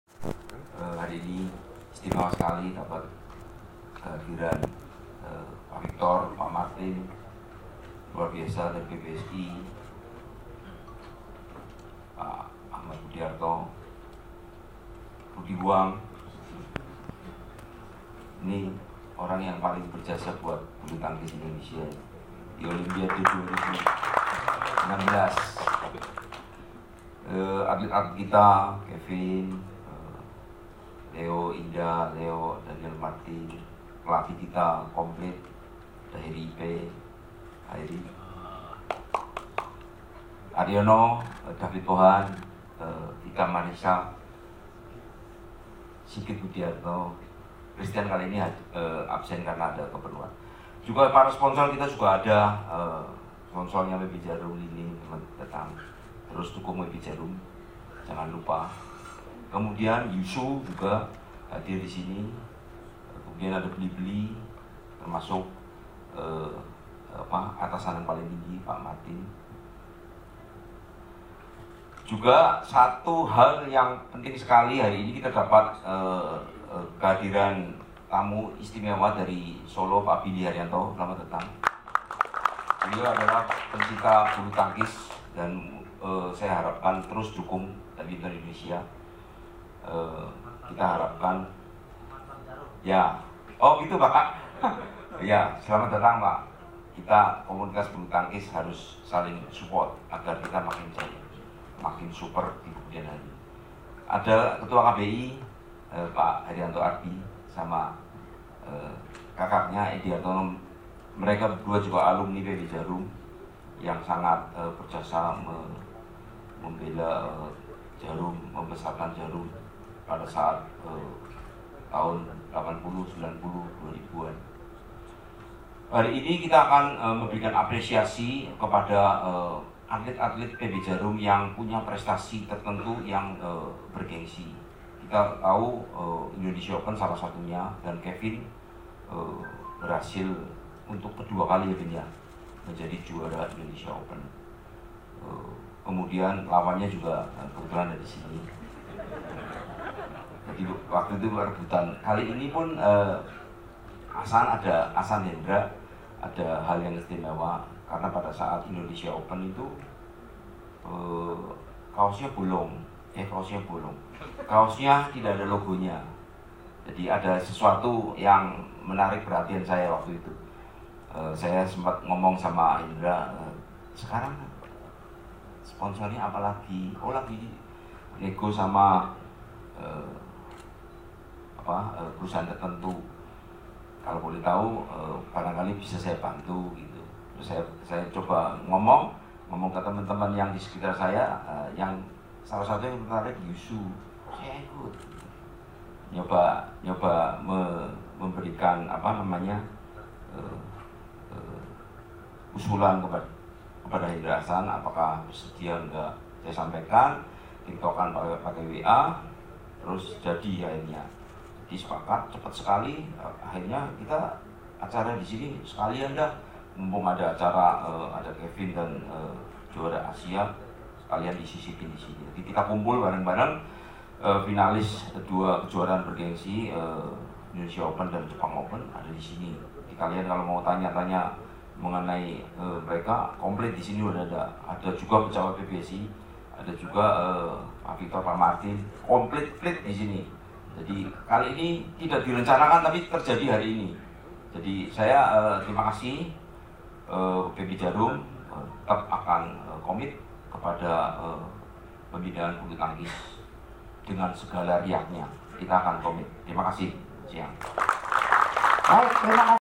Kisah dibalik Dukungan Sponsor Yuzu Untuk Ahsan/Hendra
Acara Apresiasi Juara BIO & AJC 2019 tanggal 8 Agustus 2019 di Plaza Senayan